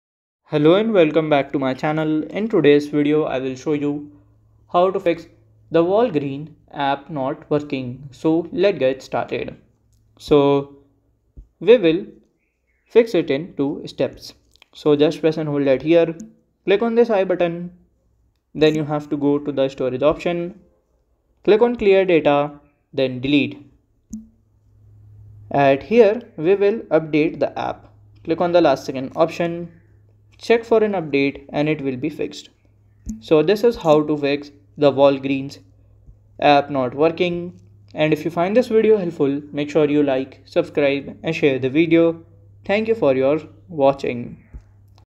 ✅ How To Fix Walgreens App Not Working (Full Guide)
Experiencing issues with your Walgreens app? Don't let technical glitches hinder your pharmacy and shopping experience! In this comprehensive guide, we'll walk you through troubleshooting steps to resolve common problems and get your Walgreens app functioning smoothly again. Whether you're facing prescription refill errors, photo printing issues, or other challenges, we've got the solutions you need. Say goodbye to interruptions in managing your health and hello to seamless experiences with Walgreens!

Key Learning Points:
• Full guide on fixing Walgreens app issues
• Step-by-step solutions for common problems
• Ensure uninterrupted pharmacy and shopping experiences with our troubleshooting tips